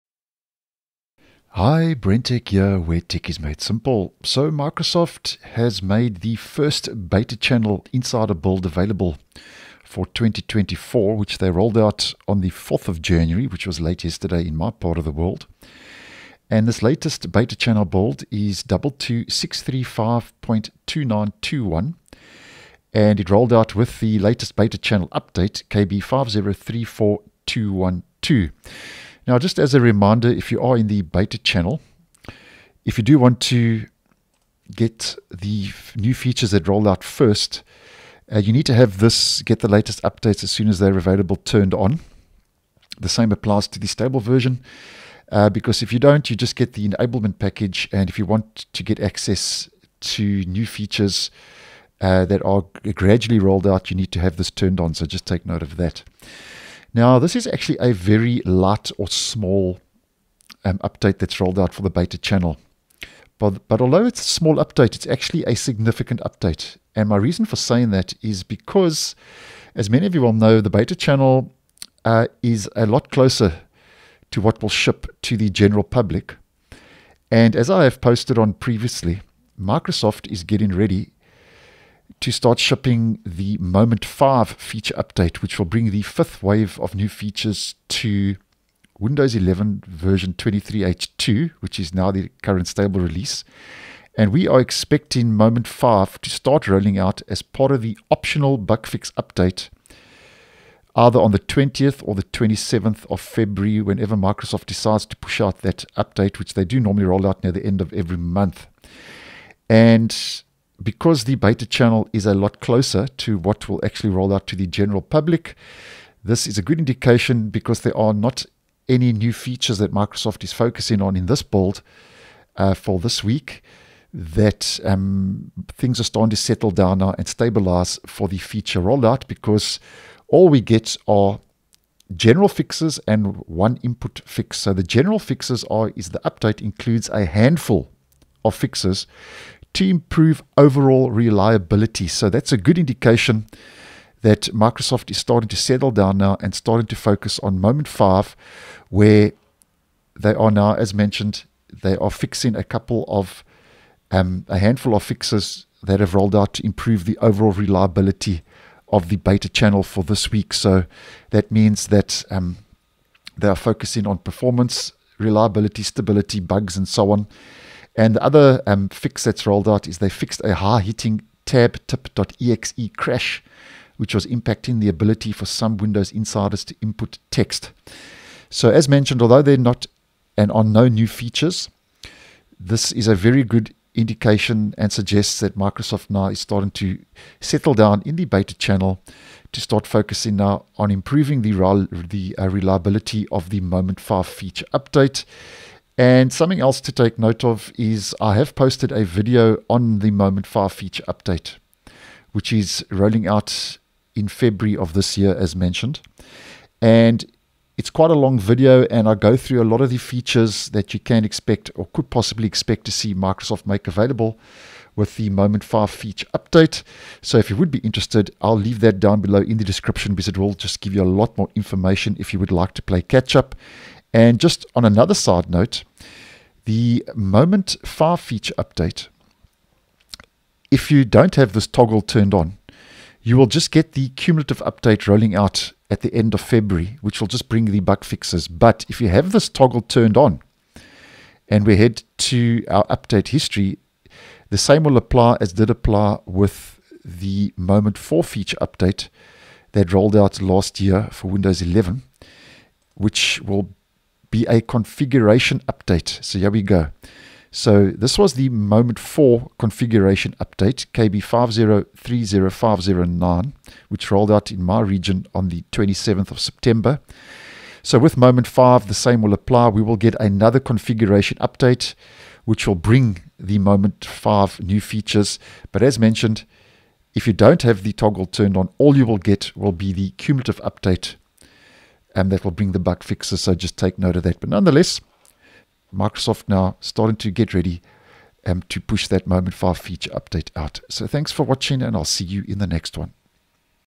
Windows 11 Build 22635.2921: Microsoft Improves the Reliability of the Moment 5 Feature Update
This latest and first Beta Channel flight (KB5034212) of the year includes a handful of fixes to improve overall reliability, indicating that Microsoft is starting to settle down for the upcoming Moment 5 feature update